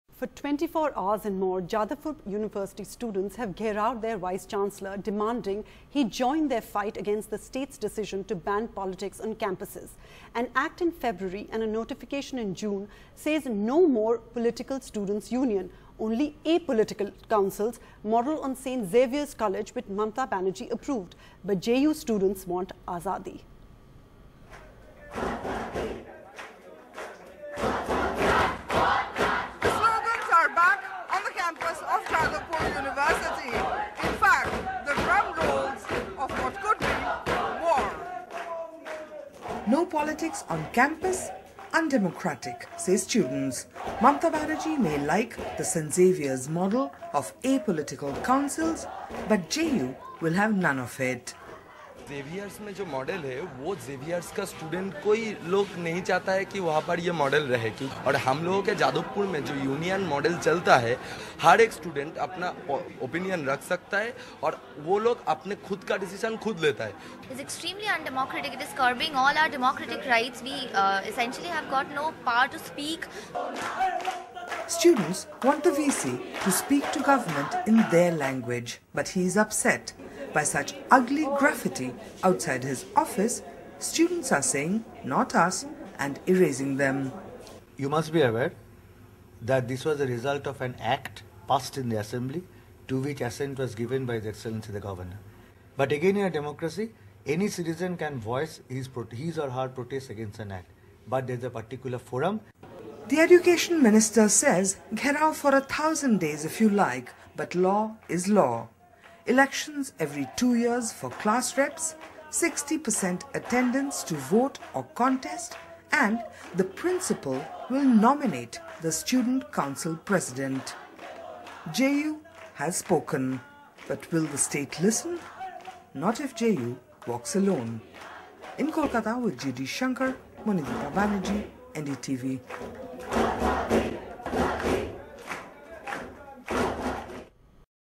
Jadavpur University Students Protest Mamata Banerjee's 'No Politics On Campus' Order
For 24 hours and more, students of Kolkata's Jadavpur University have gheraoed the vice chancellor demanding he join their fight against the state's decision to ban politics on campuses. The West Bengal Universities & Colleges (Administration & Regulation) Act passed in February and a notification in June say no more political students' unions, only apolitical councils. The model is Kolkata's best known Jesuit college and St Xavier's, which chief minister Mamata Banerjee has approved.